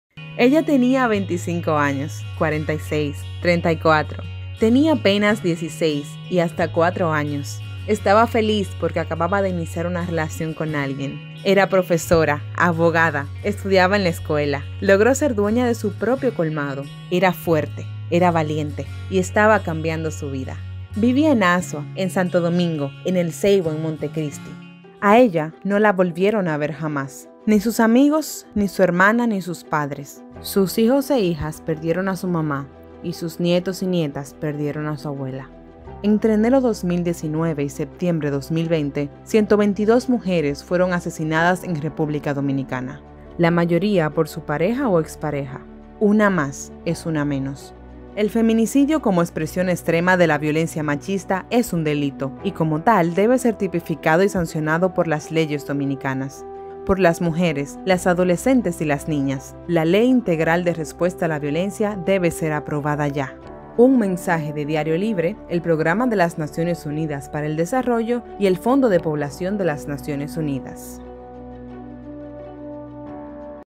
Ellas se llamaban
La violencia de género es multidimensional e impacta en capas múltiples a mujeres, niñas y comunidades enteras. República Dominicana tiene una de las tasas de feminicidio más altas de la región de América Latina y el Caribe, y si bien esta es la expresión máxima de la violencia contra mujeres y niñas, su respuesta requiere un enfoque integral, que determine los procesos necesarios para prevenir, atender a las víctimas, reparar el daño y sancionar a los agresores de manera correcta, lo que requiere la intervención de muchos sectores y un marco legal fortalecido y actualizado.

Vídeo basado en el reportaje especial "Ella se llamaban" de Margarita Cordero para Diario Libre.